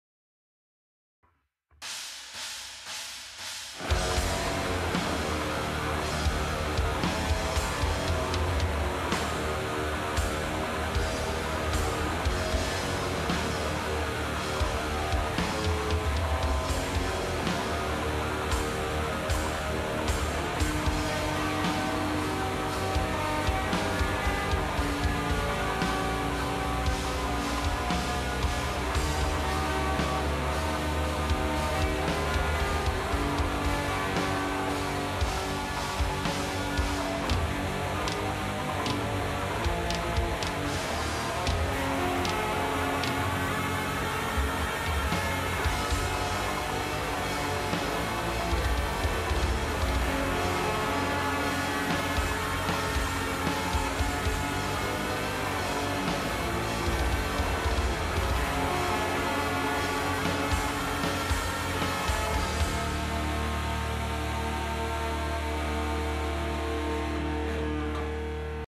Submission to Bogren Contest
here's my contribution to the layering contest from Bogren Digital.
Jackson into madrigal  fx Sweadeath preamp win a maxon std9 in front of it. Bogren's IR pack